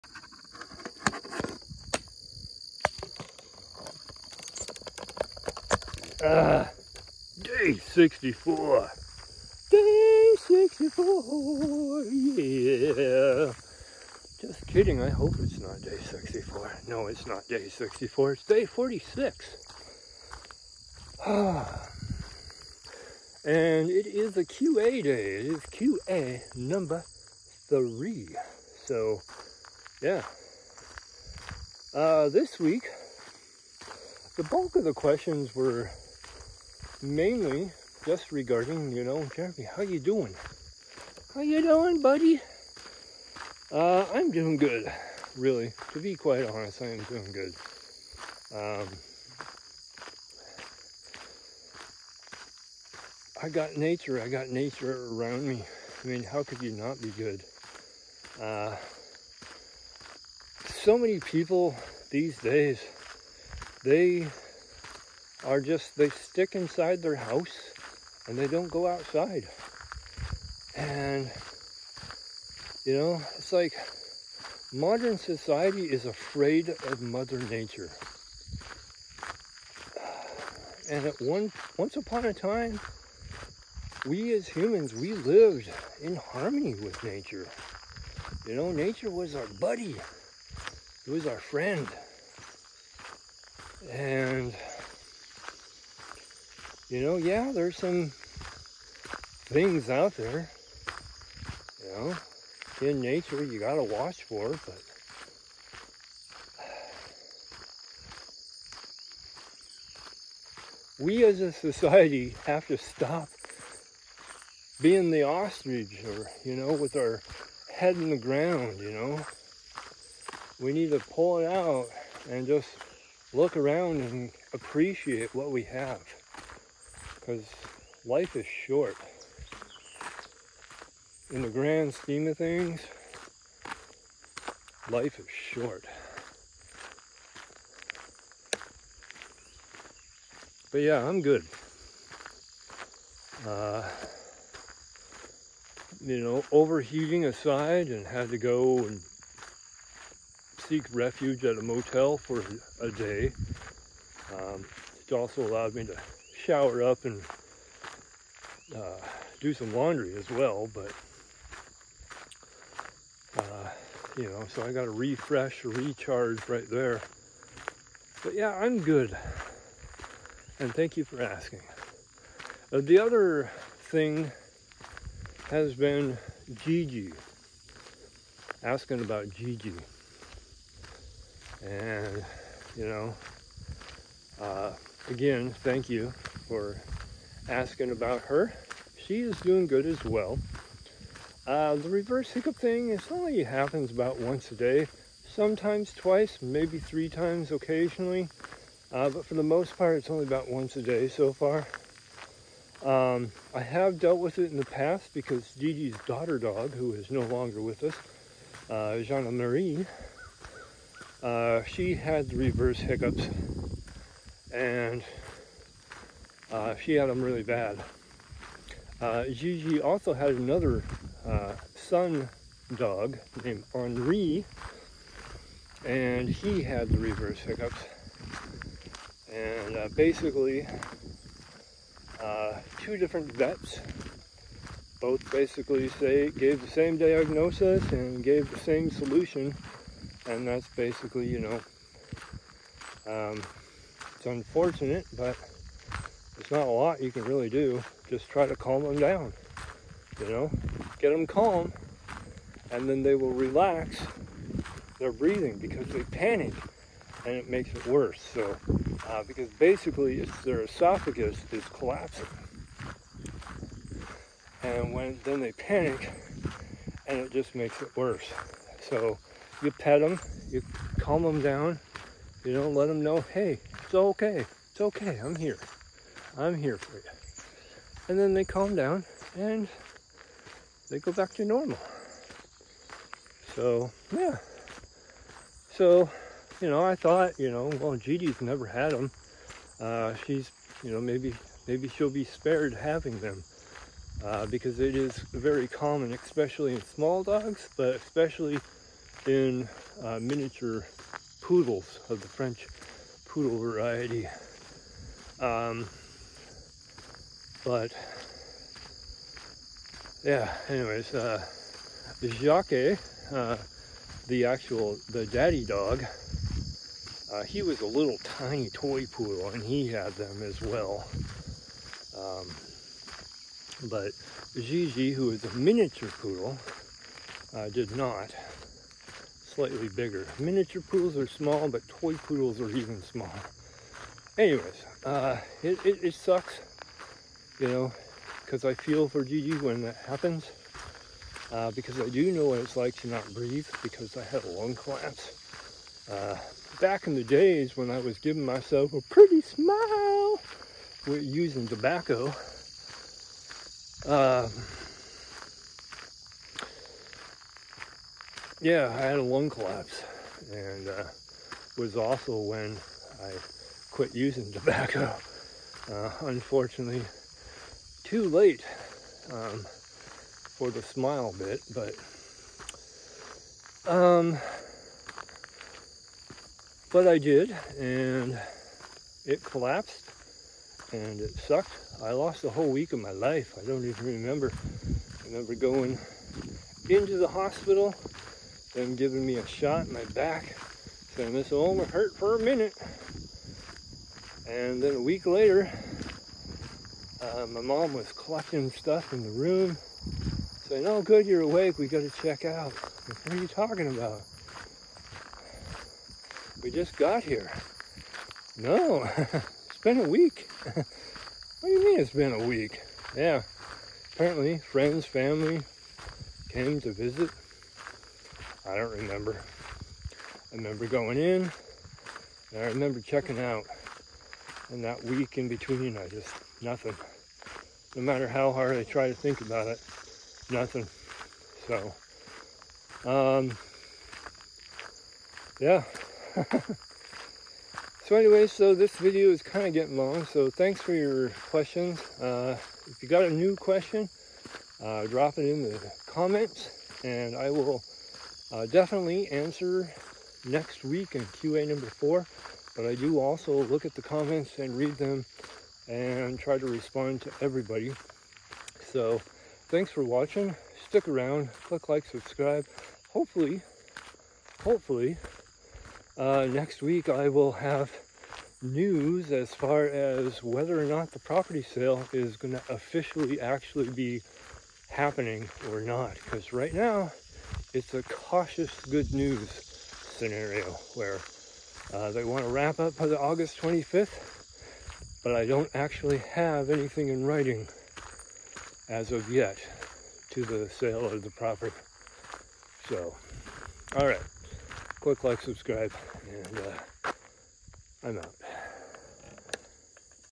Day 46:(Limboland) QA#3 A week lost
QA #3 I answer a couple questions and recall a past memory.